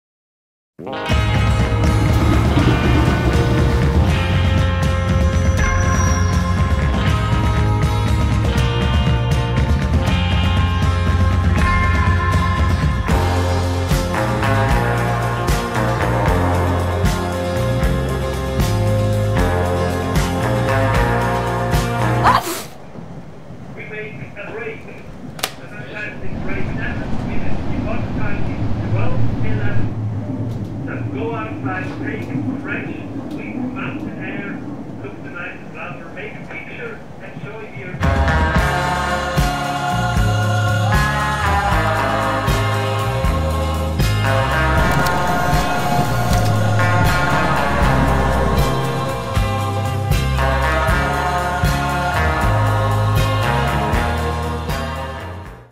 Shot of a lifetime
Travelling by Bernina Express means crossing viaducts, tunnels, mountain passes with beautiful landscapes. Travel guides promis you unforgettable experience. But what is better - to repeat a great shot or dreate your own masterpiece?